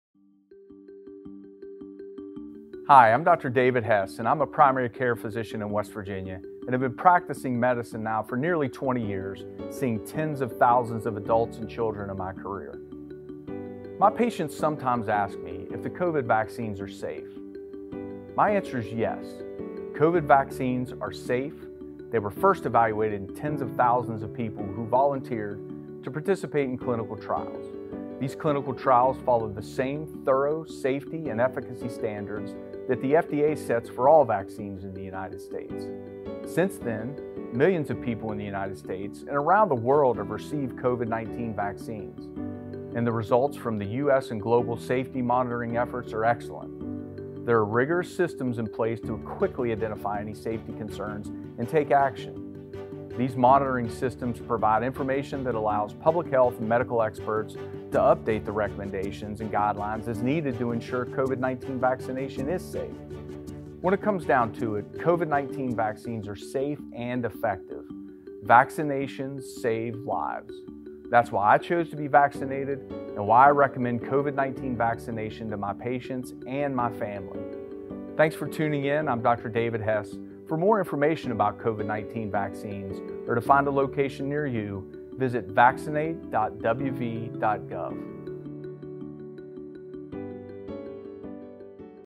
COVID-19 Vaccine Questions Answered By WV Experts: Dr. David Hess
Dr. David Hess, a primary care physician in West Virginia, answers the question: “Are the COVID-19 vaccines are safe?"